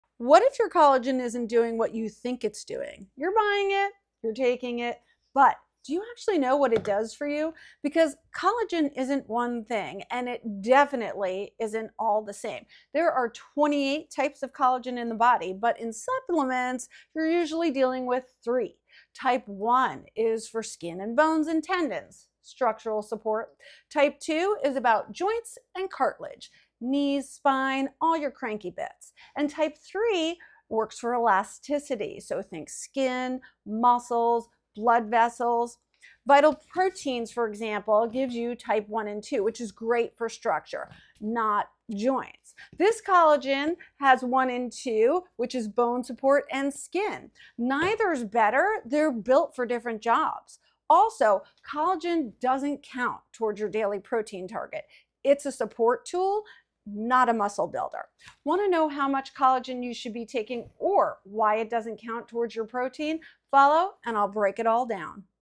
Why Your Collagen Might Not Be Working
Think your collagen is helping your joints? Maybe not.

Most powders only include Type I & III—for skin, bones, and elasticity. But if your joints still ache? You might need Type II.

In this video, I explain what collagen really does, why it doesn’t count toward your daily protein, and how to choose the right type for the results you actually want.

Follow for more supplement tips that actually make a difference—and drop a comment if you’ve ever been confused by collagen.